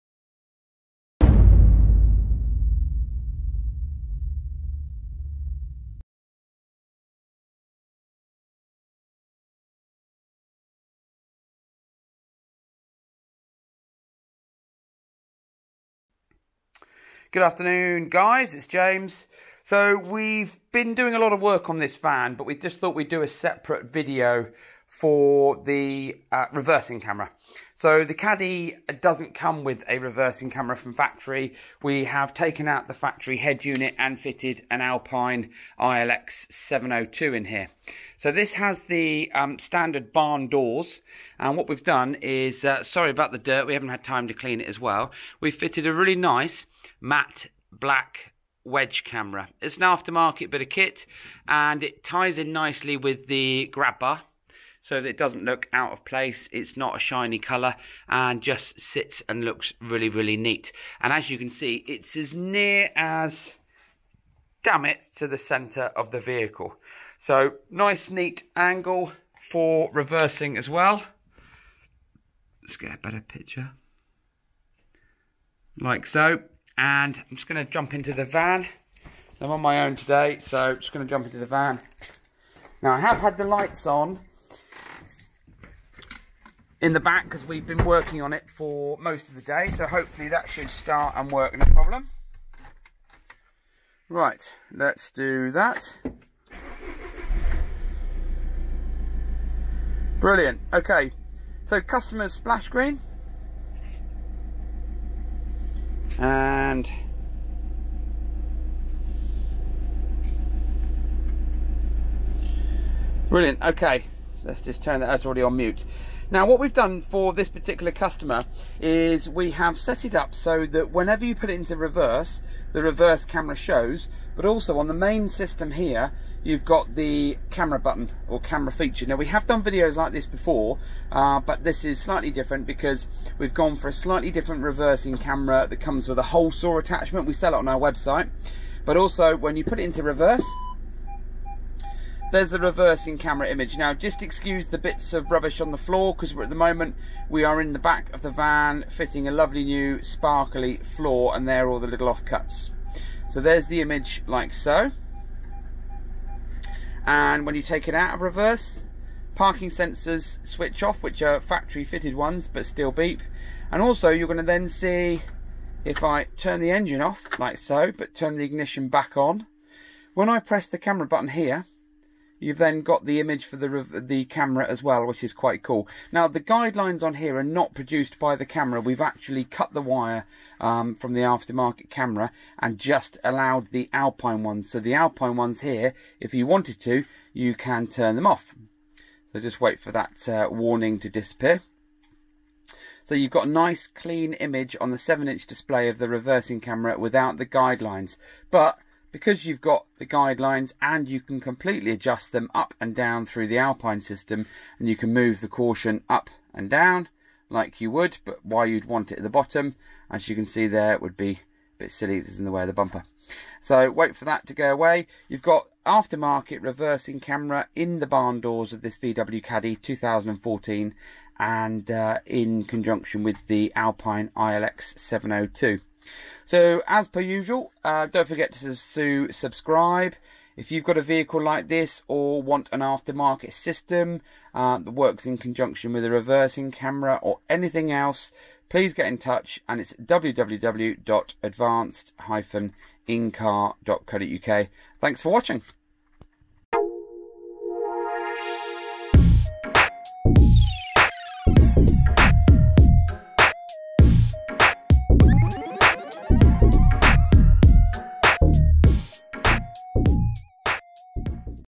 Volkswagen Caddy Reversing Camera
Volkswagen Caddy 2014 With Barn Doors. Aftermarket Wedge Reversing Camera Installed Into Alpine iLX-702D. Great Picture!

A return customer for us! Whilst we were converting, lining and carpeting his caddy we dropped in a great Wedge Camera! From factory a Barn Door Caddy never had the ability for a camera.

We pulled apart the Grab Handle and custom installed it so that it's mounted as central to the Van as possible to give you the best overall picture.

Have a watch and see how it looks whilst displaying on the Alpine iLX-702D.